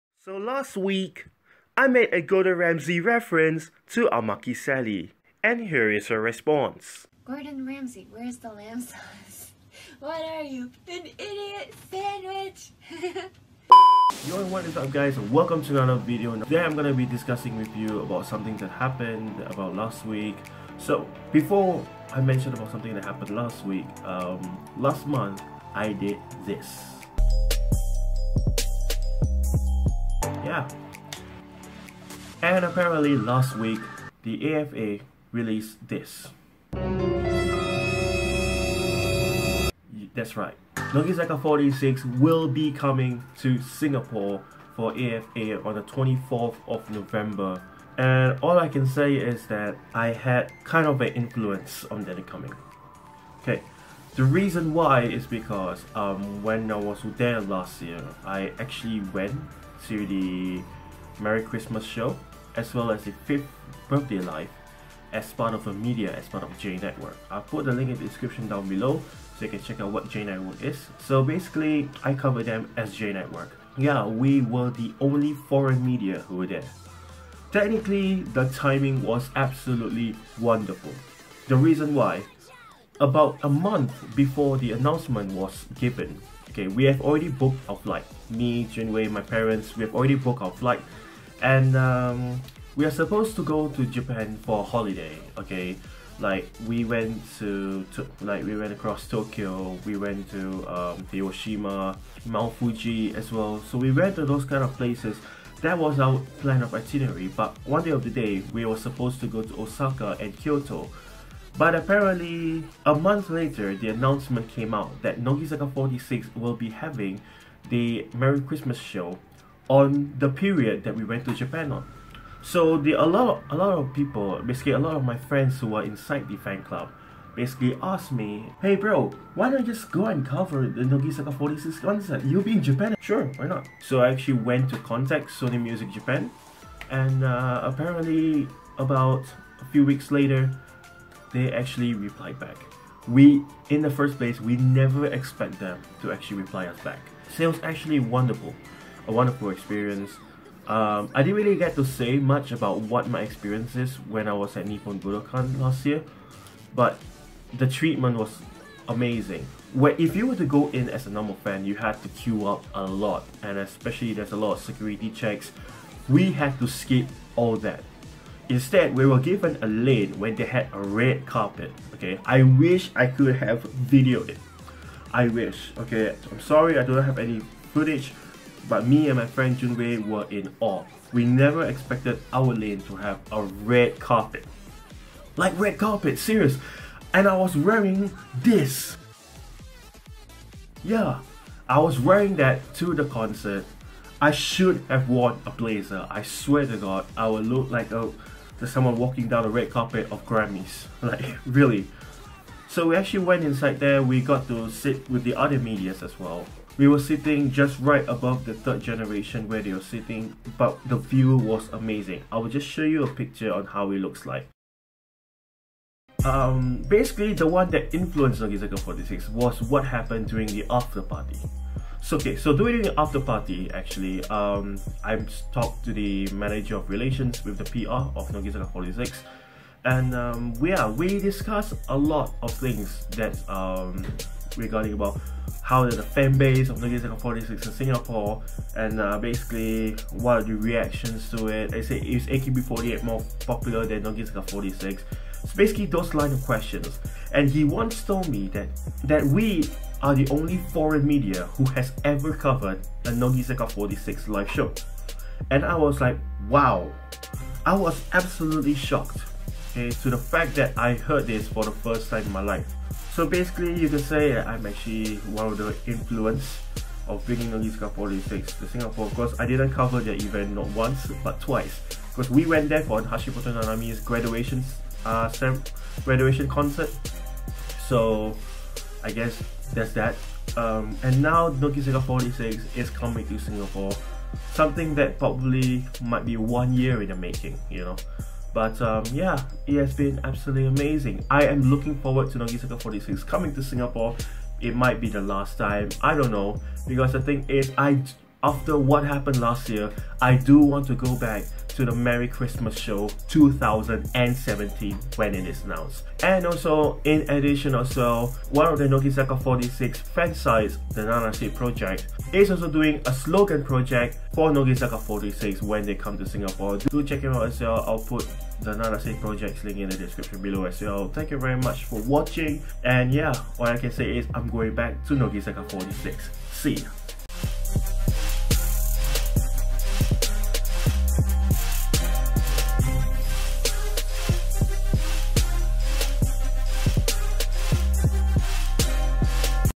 HOW I THOUGHT THAT I MADE NOGIZAKA46 COME TO SINGAPORE?!!
So a detailed explanation on how I probably brought Nogizaka46 to our great country for next month's Anime Festival Asia 2017.

Also if you are a fan of Nogizaka46, do check out the Slogan Project by TheNanaseProject! :

And yes, I'm going back to Nogizaka46 for now! :)